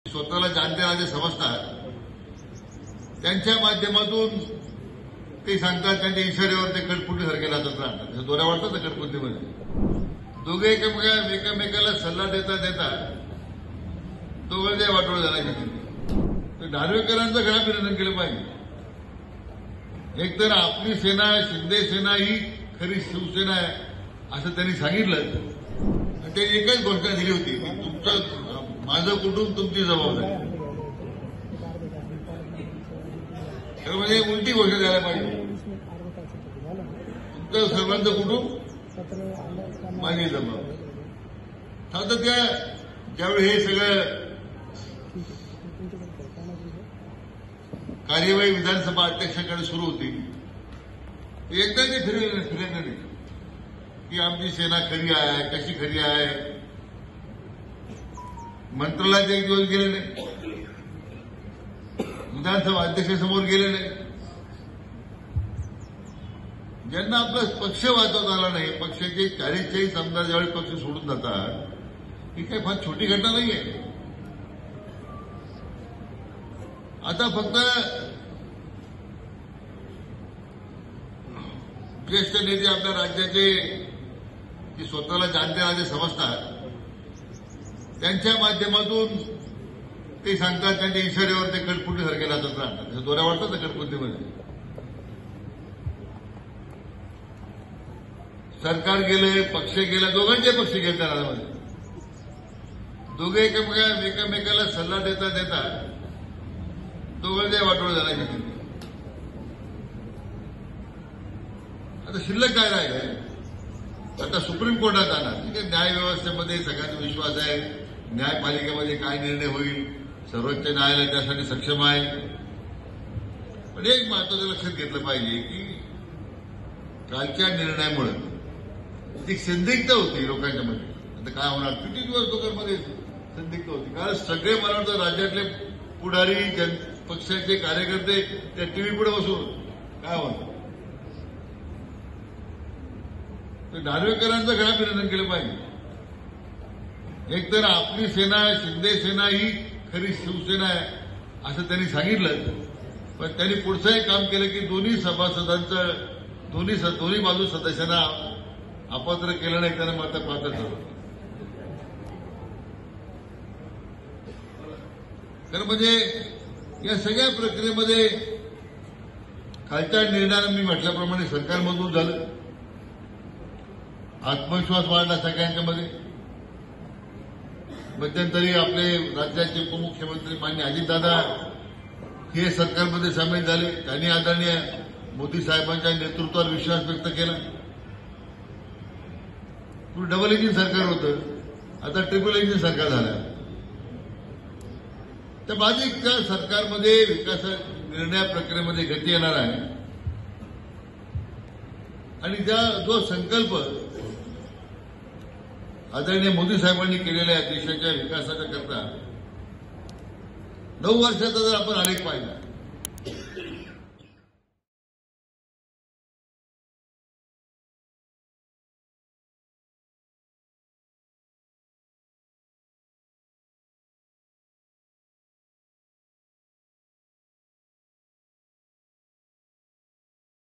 Radhakrishna Vikhe : सल्ला दिल्याने दोघांचे पक्ष संपले... | LetsUpp Marathi
विधानसभेचे अध्यक्ष राहुल नार्वेकर यांनी खरी शिवसेना एकनाथ शिंदे यांची असल्याचा निर्णय दिला. यावर बोलताना राज्याचे महसूल मंत्री राधाकृष्ण विखे पाटील यांनी उद्धव ठाकरे आणि शरद पवार यांच्यावर कडाडून टीका केली.